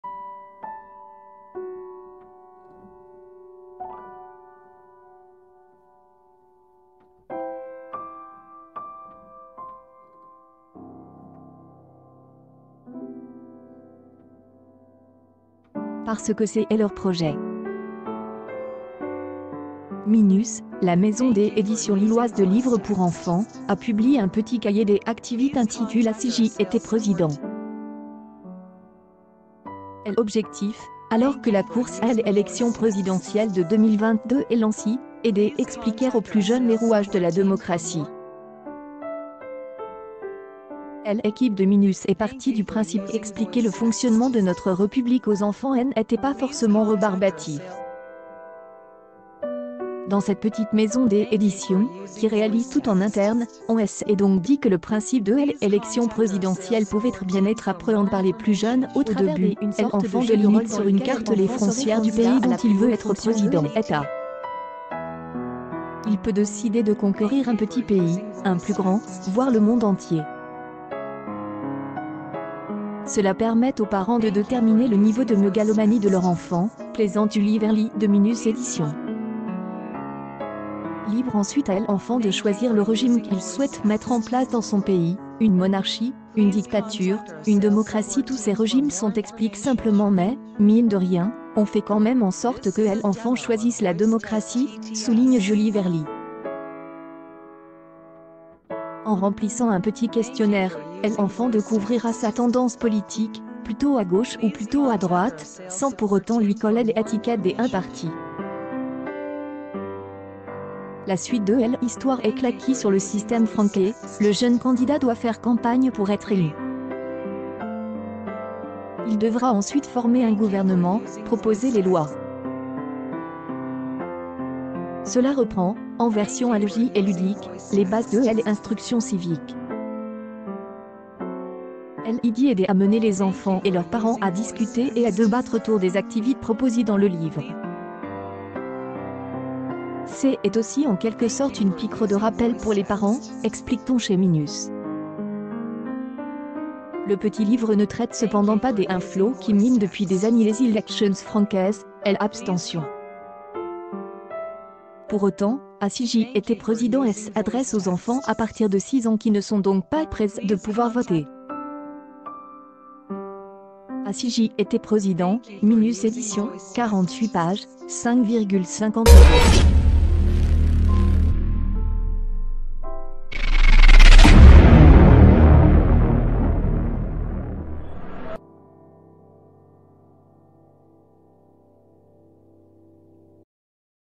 Lille : « Ah si j’étais président », le livre qui va faire aimer la politique aux enfants
,Enfant,Livres,Election présidentielle,
Lille : « Ah si j’étais président », le livre qui va faire aimer la politique aux enfants Parce que c’est leur projet. Minus, la maison d’édition lilloise de livres pour enfants, a publié un petit